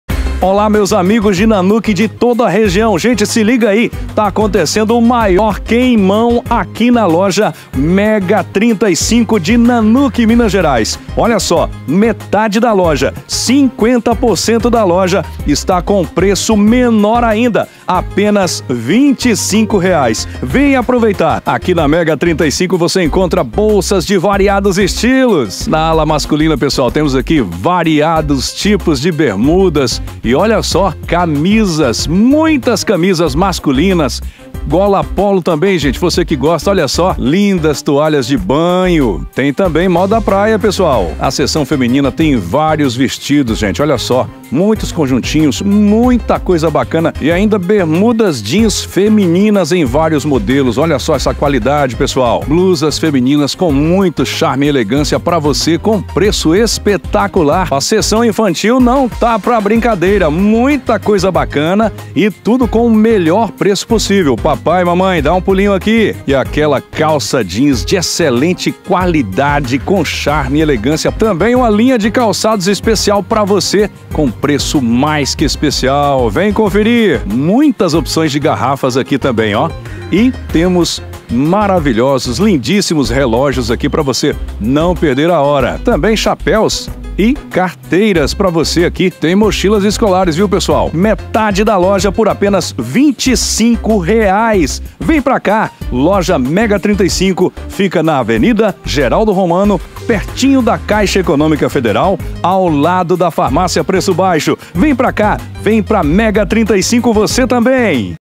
QUEIMA NA MEGA 35 DE NANUQUE/MG
-- METADE DA LOJA COM PREÇO ÚNICO DE R$ 25,00.
QUEIMA IMPERDÍVEL NA LOJA MEGA 35 DE NANUQUE/MG.
DEIXE CURTIDA, COMENTE, REPASSE.